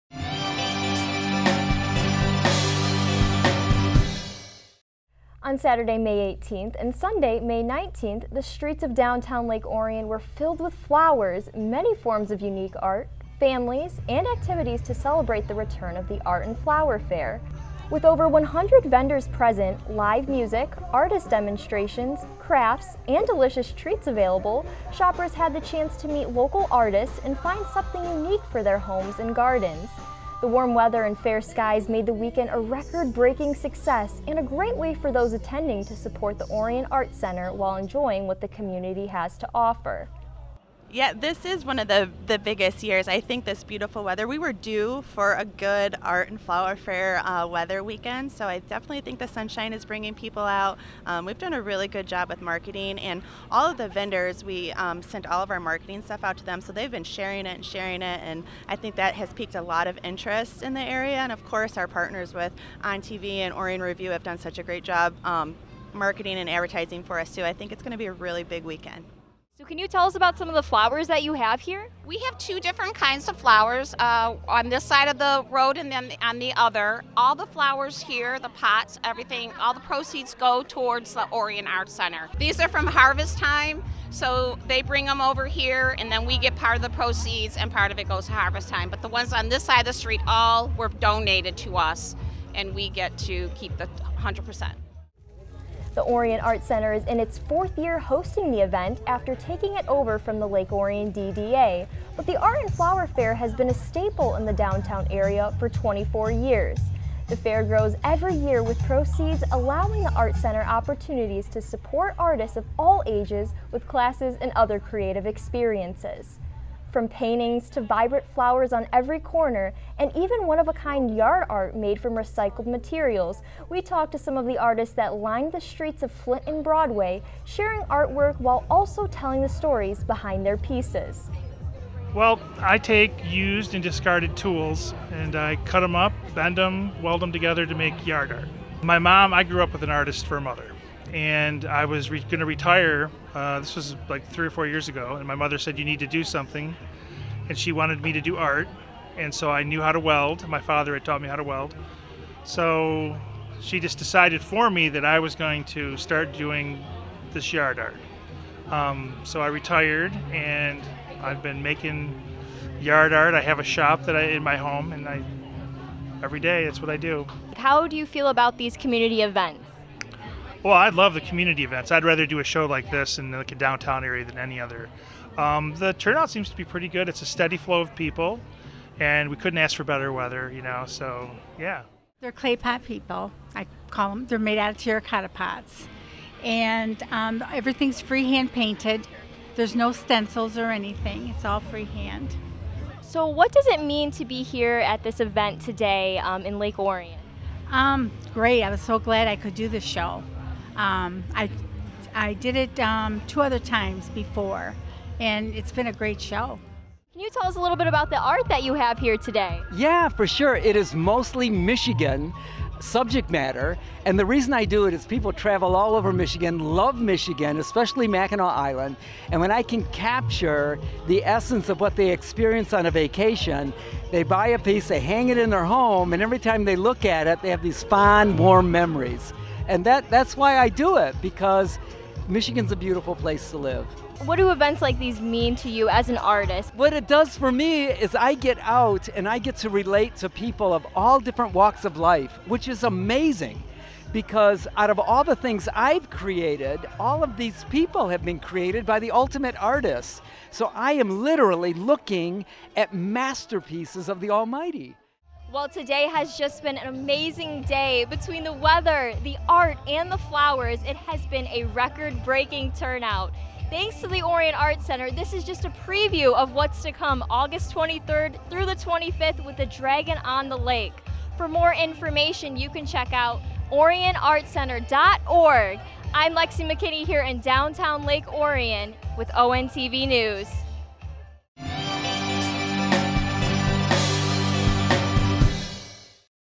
ONTV News Break: OAC Art & Flower Fair 2024
On May 18 & 19, 2024, the Orion Art Center hosted its annual Art & Flower Fair on the streets of downtown Lake Orion.